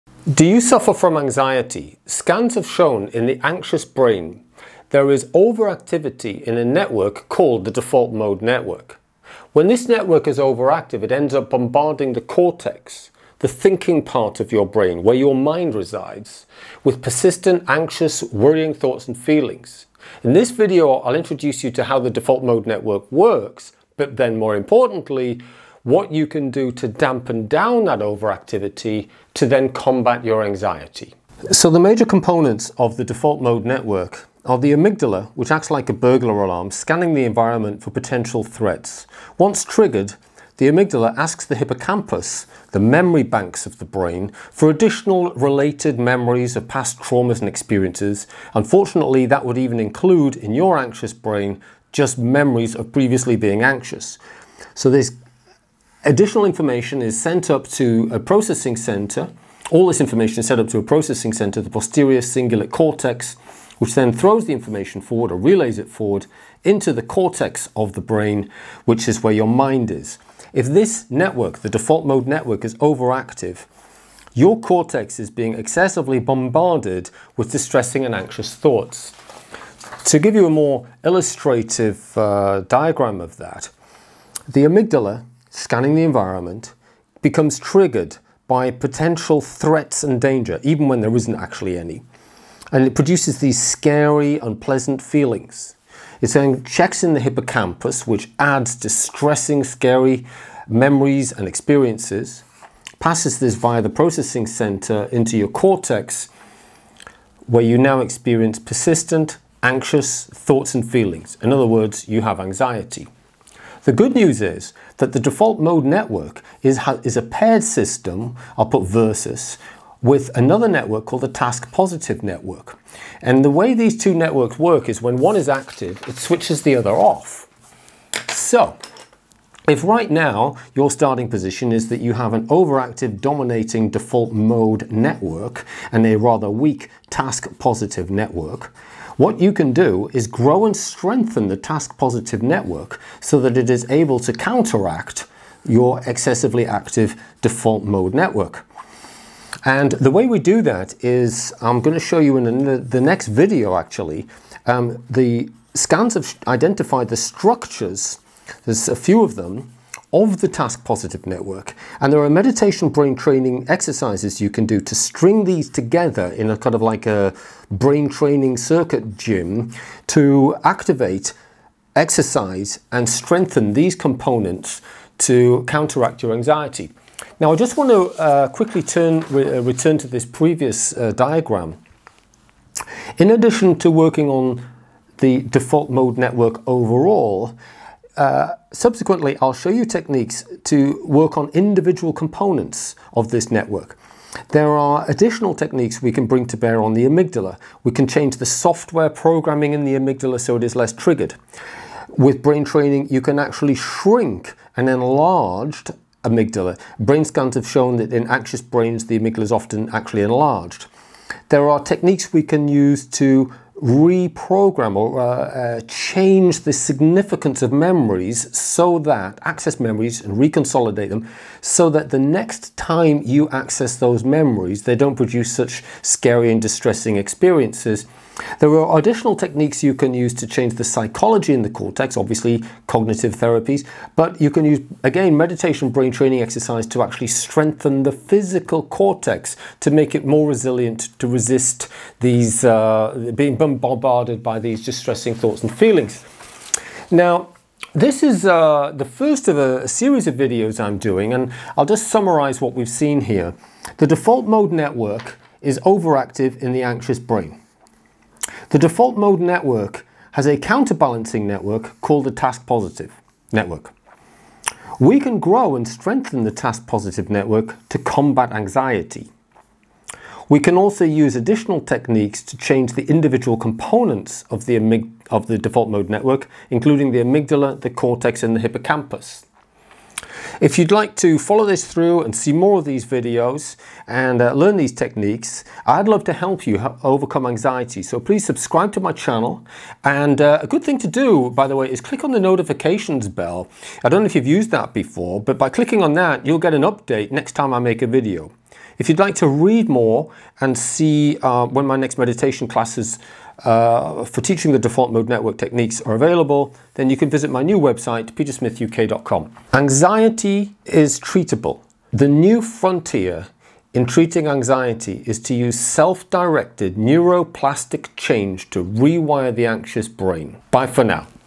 How to Overcome Anxiety Part 1
In the anxious brain scans show overactivity in a neurocircuit called the default mode network.  We can combat anxiety by re-wiring our brain circuits with brain training.

If you’d like my professional help to get an individual prescription of supplements, dietary advice, brain training exercises and other techniques.